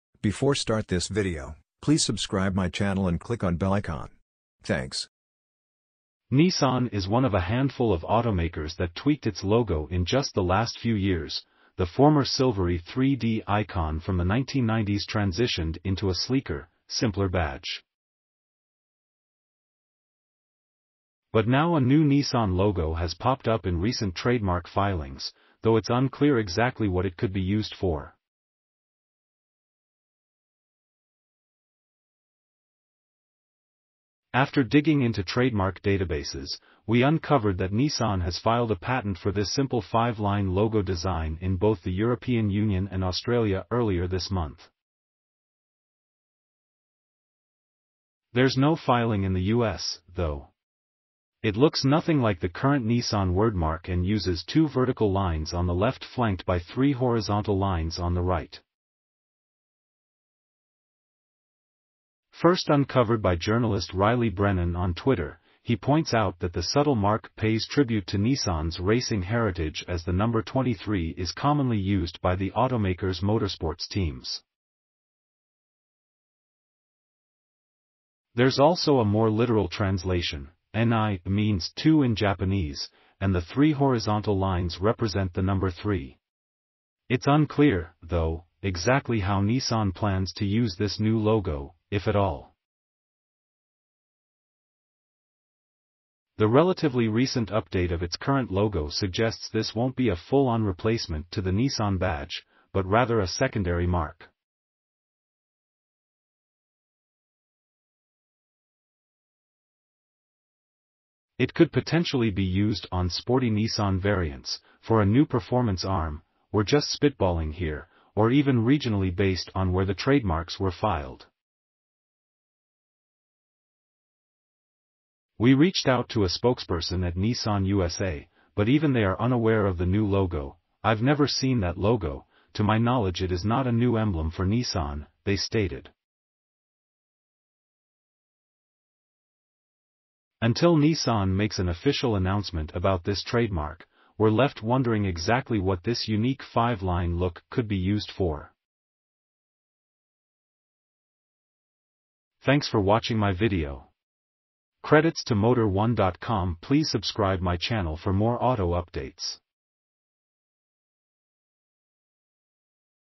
Nissan Trademark Filings Show Simple New Logo, But For What?
Nissan is one of a handful of automakers that tweaked its logo in just the last few years; the former silvery 3D icon from the 1990s transitioned into a sleeker, simpler badge. But now a new Nissan logo has popped up in recent trademark filings, though it’s unclear exactly what it could be used for. After digging into trademark databases, we uncovered that Nissan has filed a patent for this simple five-line logo design in both the European Union and Australia earlier this month. There's no filing in the US, though. It looks nothing like the current Nissan wordmark and...